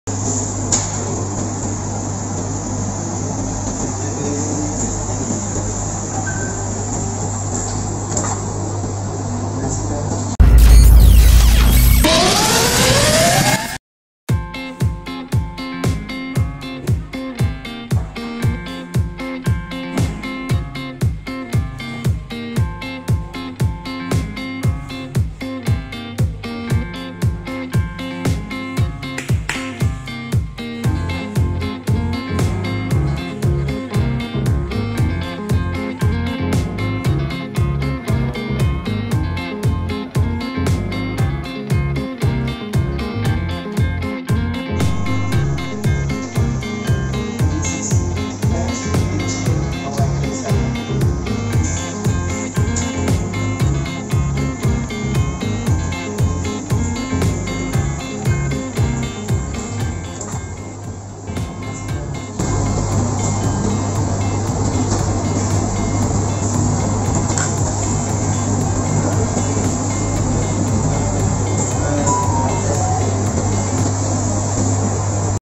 Mengolah Ikan salmon Agar Awet disimpan
Ikan yang tidak tepat dalam pengolahan akan menghilangkan Khasnya.

dalam video singat ini memperlihatkan bagaimana Ikan jenis Salmon di Simpan dengan cara yang terdapat dalam video ini.